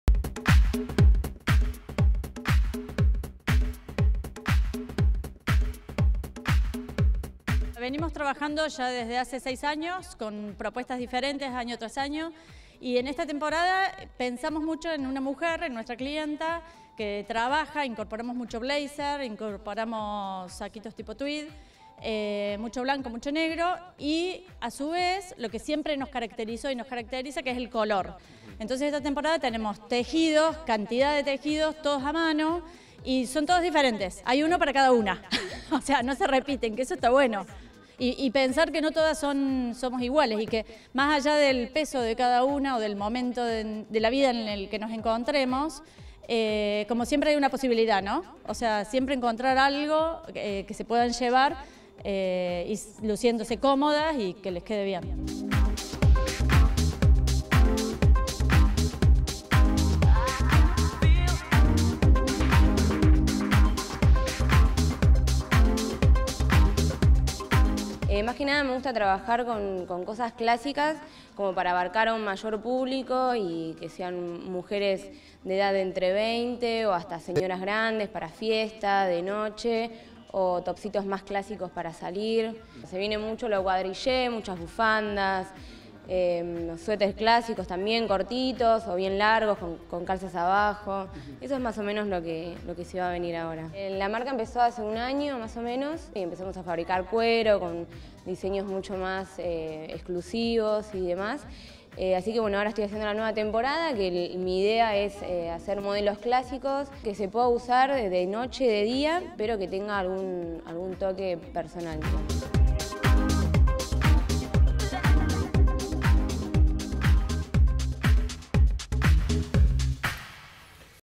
LA MODA PARA EL OTOÑO/INVIERNO
En una recorrida por locales del centro Comercial
de Martínez, les mostramos cuáles son las tendencias
y qué ropa está de moda en esta época del año.